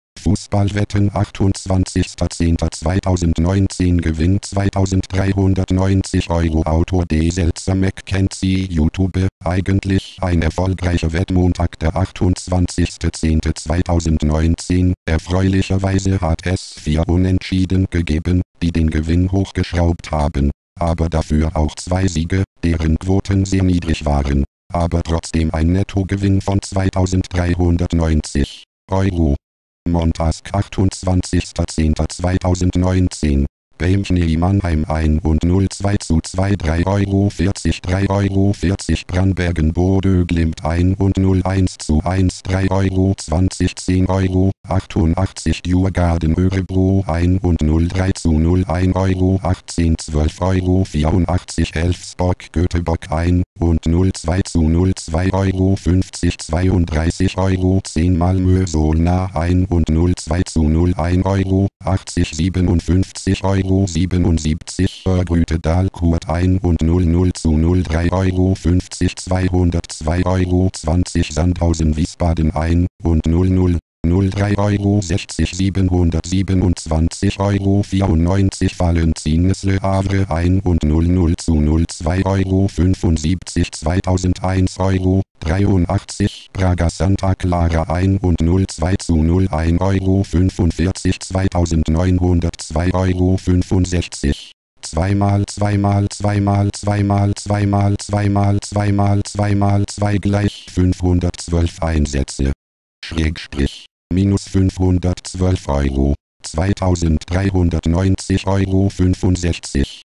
Fussballwetten 28.10.2019 Gewinn € 2.390.—
Author D. Selzer-McKenzie
Eigentlich ein erfolgreicher Wett-Montag der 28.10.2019, erfreulicherweise hat es 4 Unentschieden gegeben, die den Gewinn hochgeschraubt haben,. Aber dafür auch 2 Siege, deren Quoten sehr niedrig waren. Aber trotzdem ein Nettogewinn von 2.390.—Euro.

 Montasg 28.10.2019
BayMchnII Mannheim 1 und 0 2:2 3,40 € 3,40 €
BrannBergen BodöGlimt 1 und 0 1:1 3,20 € 10,88 €
Djurgarden Örebro 1 und 0 3:0 1,18 € 12,84 €
Elfsborg Göteborg 1 und 0 2:0 2,50 € 32,10 €
Malmö Solna 1 und 0 2:0 1,80 € 57,77 €
Örgryte Dalkurd 1 und 0 0:0 3,50 € 202,20 €
Sandhausen Wiesbaden 1 und 0 0:0 3,60 € 727,94 €
Valenciennes LeHavre 1 und 0 0:0 2,75 € 2.001,83 €
Braga SantaClara 1 und 0 2:0 1,45 € 2.902,65 €
 2x2x2x2x2x2x2x2x2 = 512 Einsätze   ./. -512,00 €
     2.390,65 €